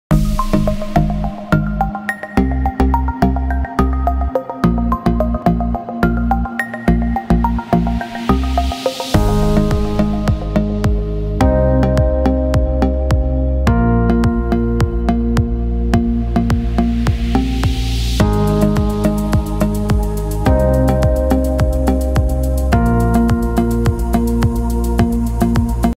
How to change violet buttercream to burgundy
Quick tutorial to change light purple shade buttercream into burgundy shade.  Add 1 drop of black 1 drop of brown then continue to add burgundy icing color until desired shade is achieved.

wilton icing burgundy
wilton color icing brown
Chefmaster coal black
violet buttercream